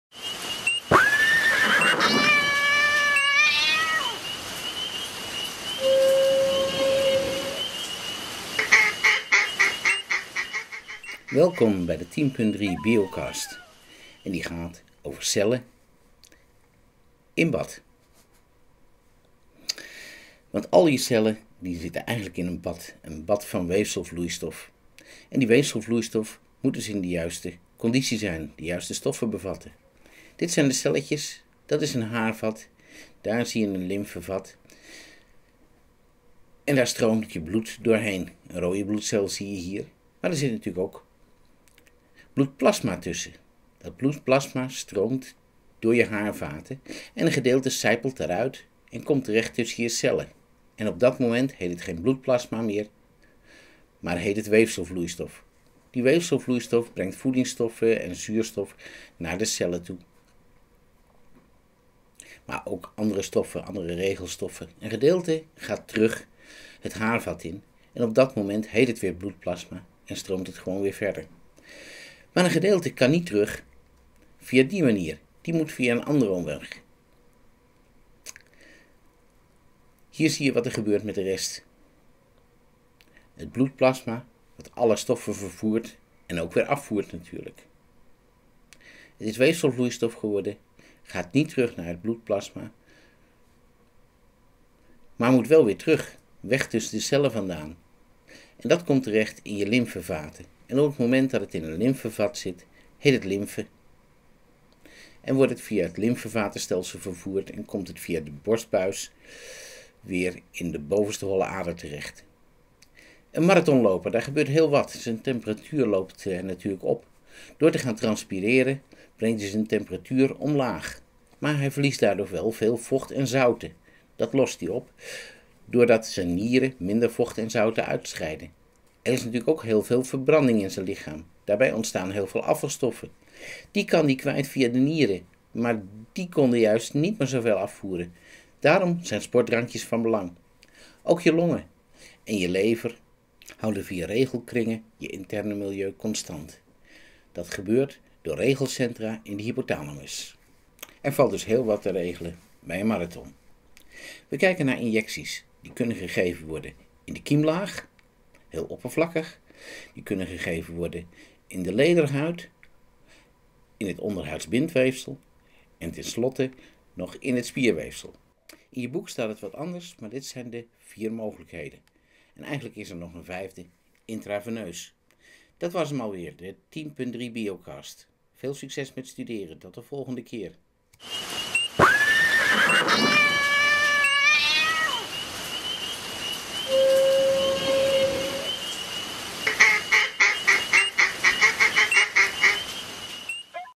10.3 biocast
Een podcast over hoofdstuk 10.3 van Nectar voor V5